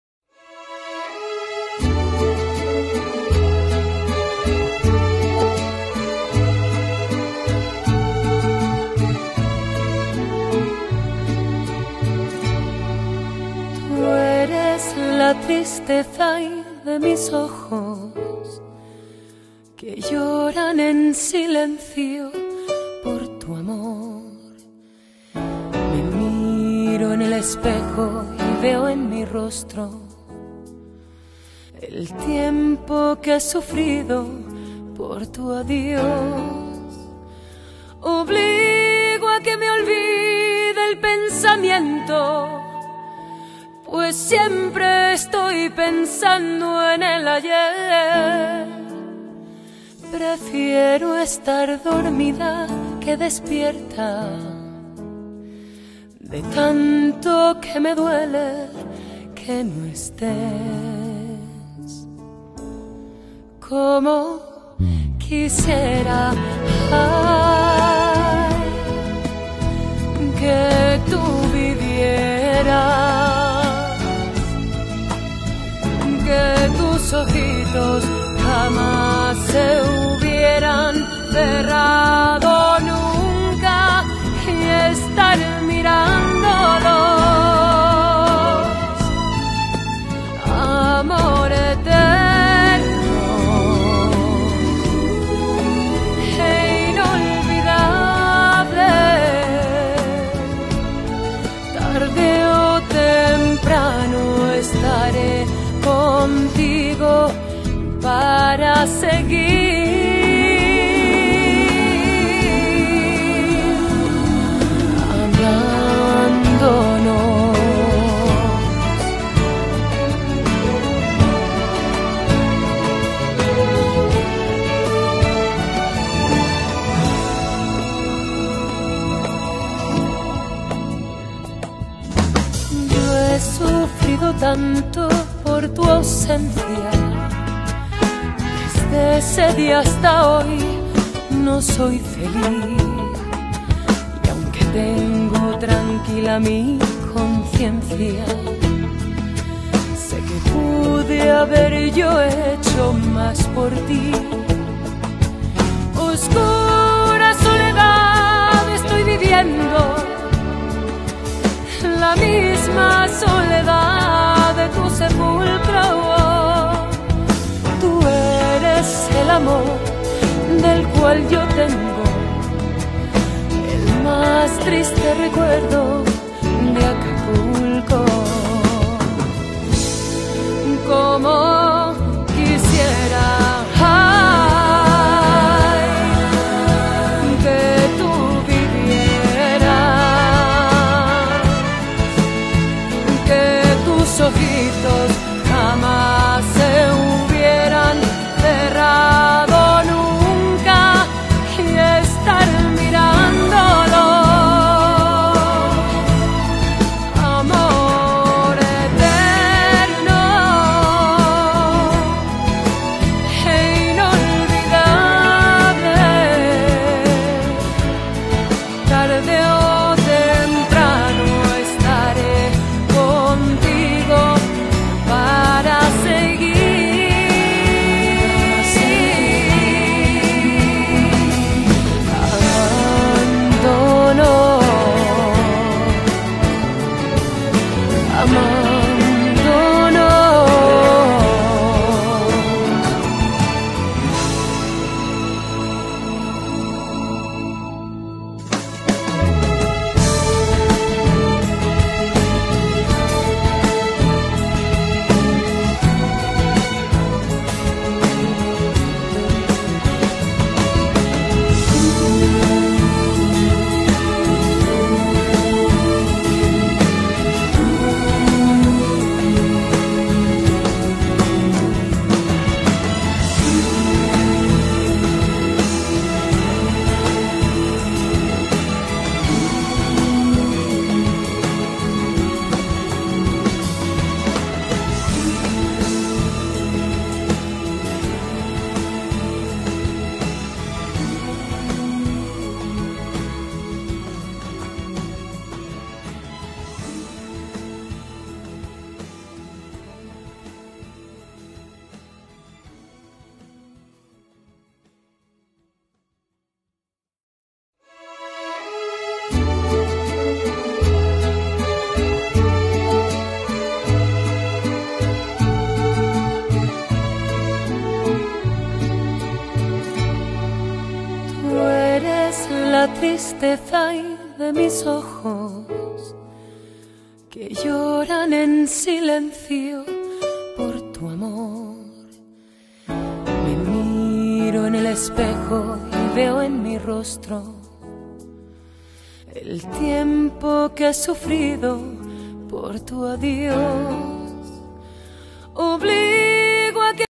Mamita amada♥️
Recordando a mi mamita amada en el día de su cumpleaños ♥️